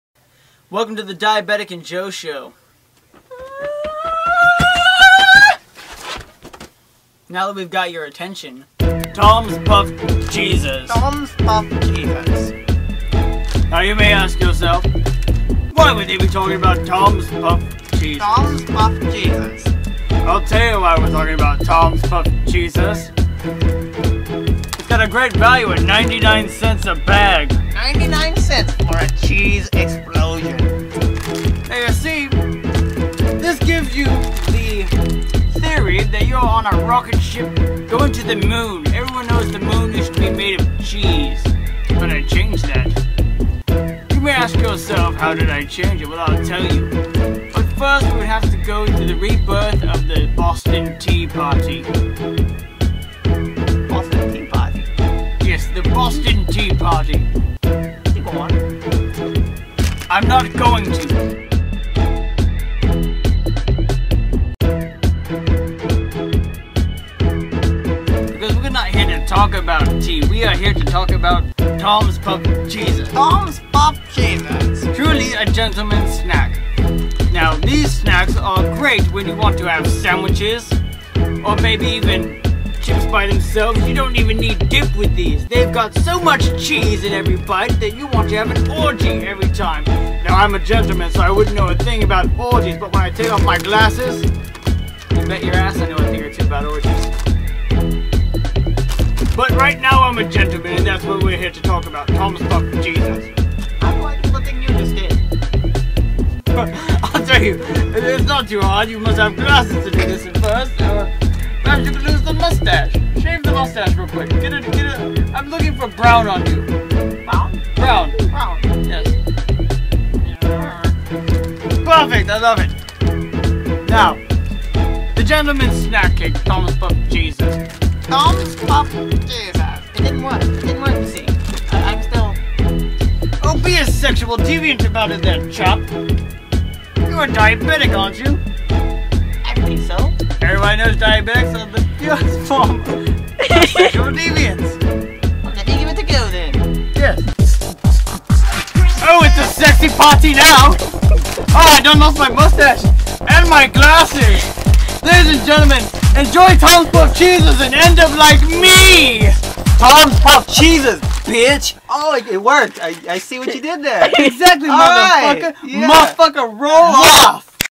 Tom's Puffed Cheezers
Diabetic & Joe take it upon themselves to inform the uninformed about the pure, unadulterated, vag-poundingly incredible snacks known as "Tom's Puffed Cheezers".